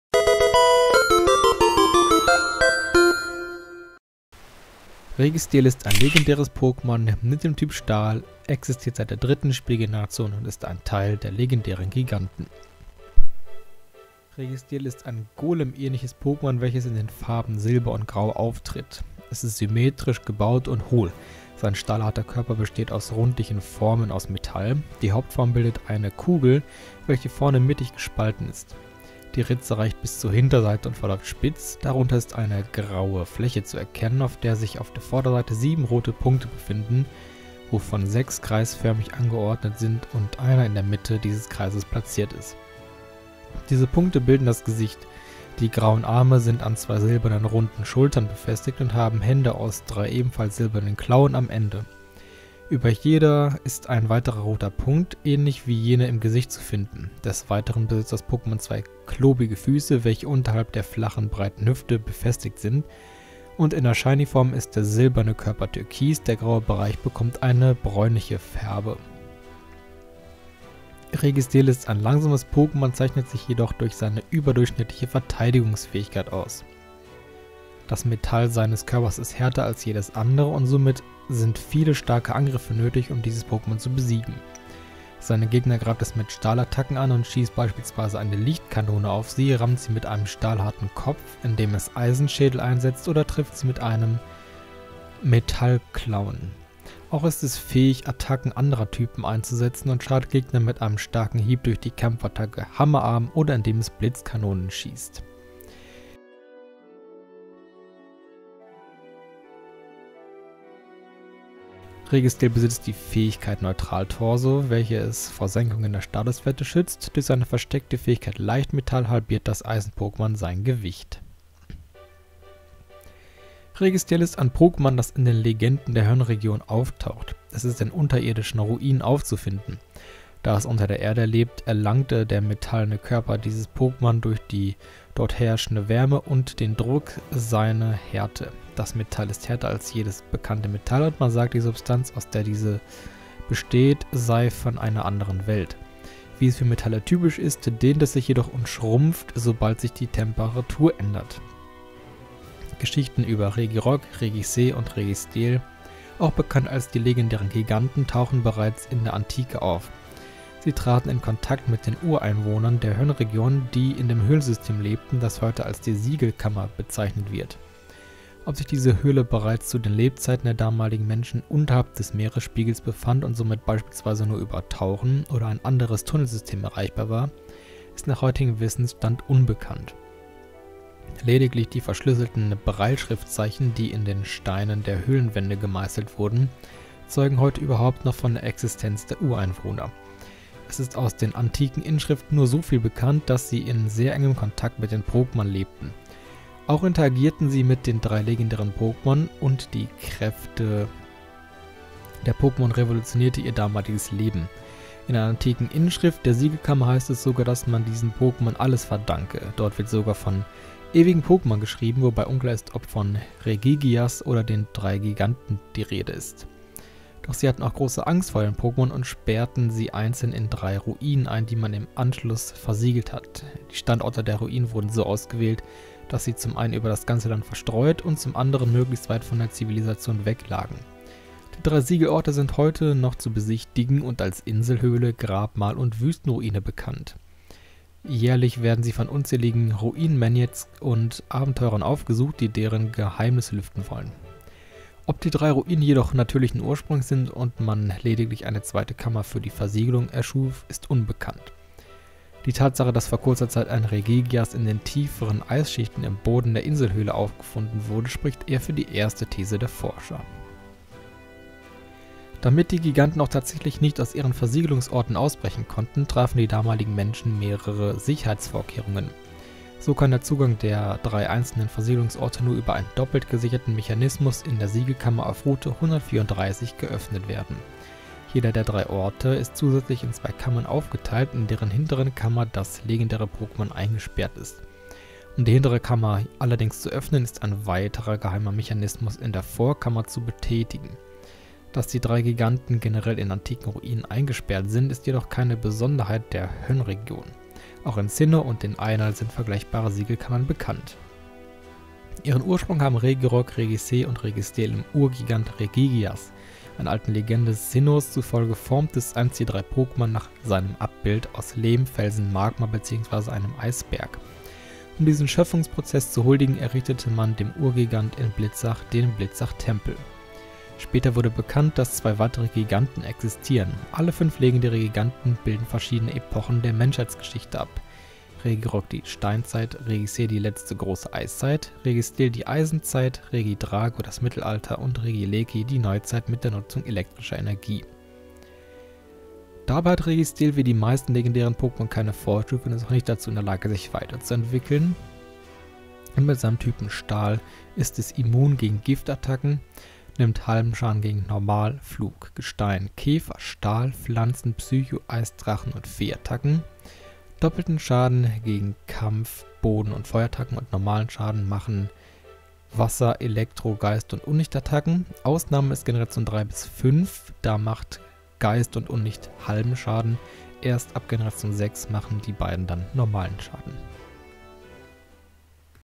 Das Pokémon #379 | Registeel
▶️Worum gehts in diesem Video:

Ich stelle euch das Pokémon Registeel vor. Dabei schauen wir auf das Aussehen, Attacken, Verhalten, Lebensraum, Entwicklung und Typ Schwächen. Viel Spaß :D

0:00 Intro
0:14 Aussehen
1:10 Attacken & Fähigkeiten
2:03 Verhalten & Lebensraum
5:38 Entwicklung
5:50 Typ Schwächen